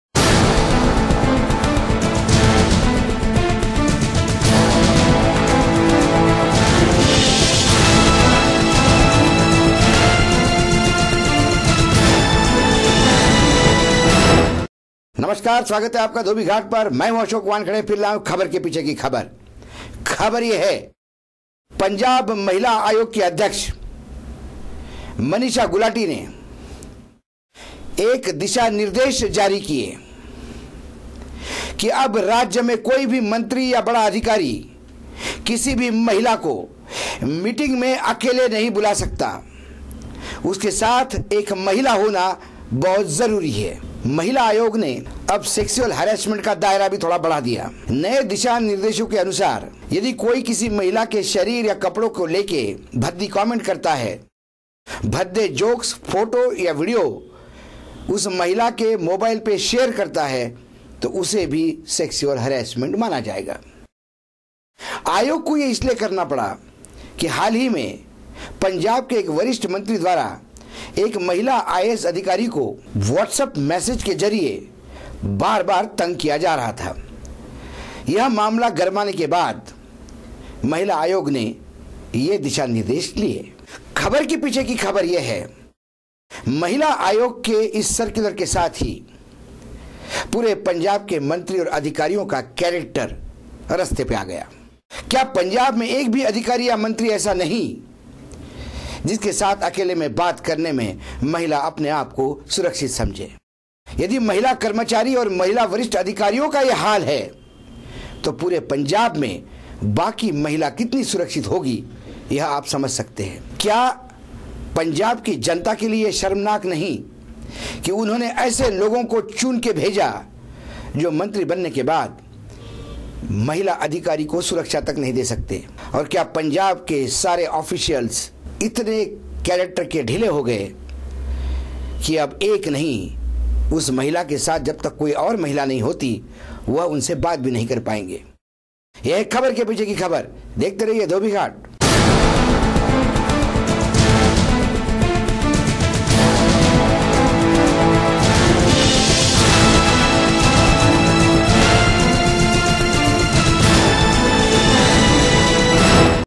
मंत्री और अधिकारी महिला के लिए खतरा
पंजाब महिला आयोग की अध्यक्ष ने सरकार के लिए नए दिशा निर्देश जारी किए. नए दिशा निर्देश के अनुसार कोई भी मंत्री या अधिकारी अकेली महिला को मीटिंग के लिए नहीं बूला सकते , उस महिला के साथ एक और महिला होना जरूरी है. महिला आयोग ने सेक्सुअल हरासमेंट का दायरा भी बढा दिया. भद्दे जोक्स, फोटो या वीडियो मोबाइल पर शेयर करने पर भी सेक्सुअल हरासमेंट माना जायेगा. क्या पंजाब में मंत्रियों और अधिकारियों का कैरेक्टर इतना नीचे गिर गया है ? क्या पंजाब में महिला मंत्री और अधिकारियों से सुरक्षित नहीं ? क्या इससे सेक्सुअल हरासमेंट रुकेगा ? इन तमाम सवालों  के जवाब जानने के लिए देखिए खबर के पीछे की खबर धोबी घाट पर अशोक वानखेड़े के साथ.

Punjab women Commission has issued the guidelines for the Ministers and senior officials of the state that they will not call any women staff alone for a meeting. There has to be minimum 2 women togather. It has also brought sharing of vulgar jokes , photos and Vidoes with female staff as sexual harrashment.  Are officials and Minister in Punjab charecterless ? Will this stop sexual harrashment in the state ? Are women safe in Punjab ? To get answers to these questions watch Khabar ke piche ki khabar on Dhobighat with Ashok Wankhade.